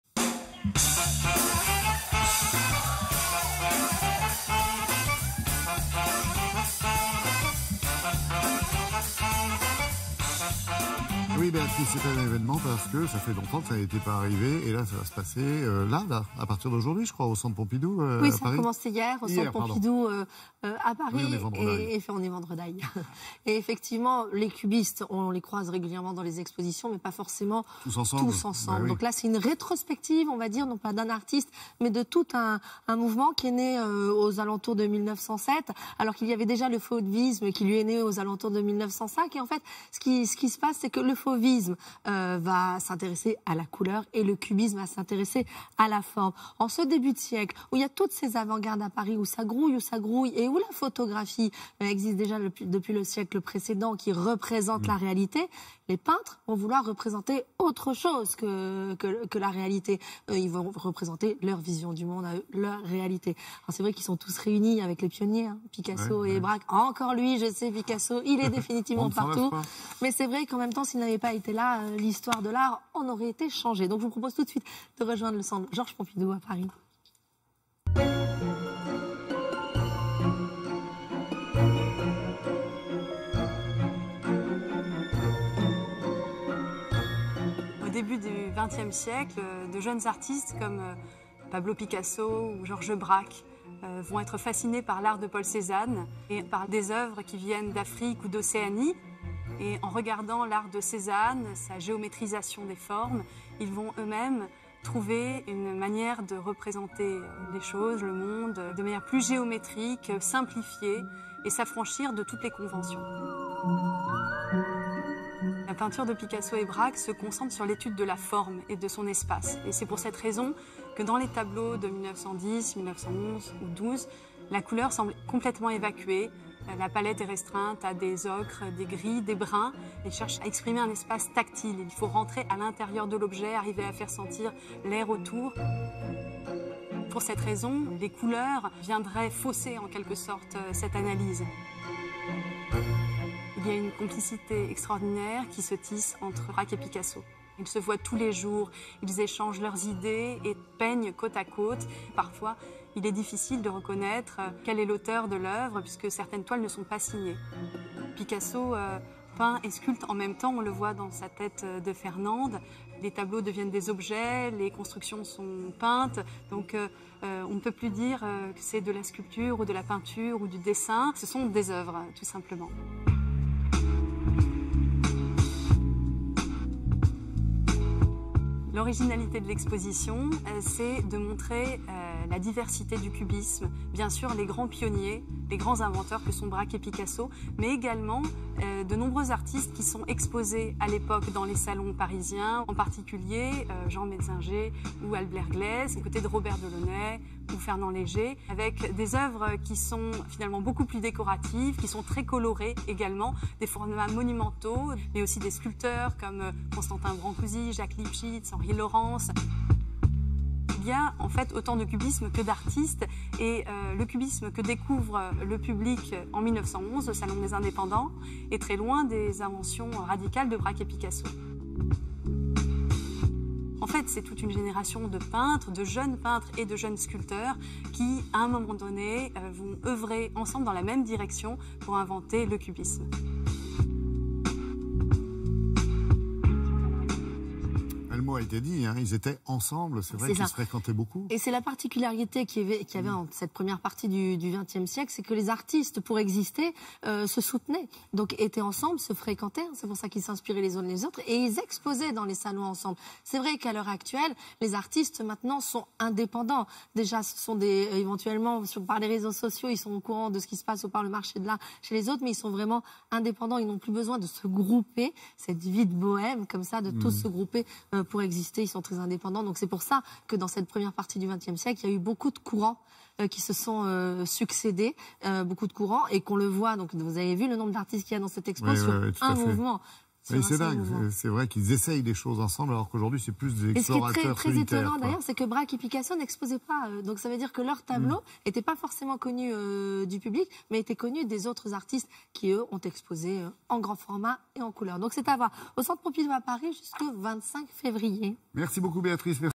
Expo - Le cubisme au Centre Pompidou
Le Centre Pompidou propose une traversée inédite et un panorama complet de l’un des mouvements fondateurs de l’histoire de l’art moderne : le cubisme (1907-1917).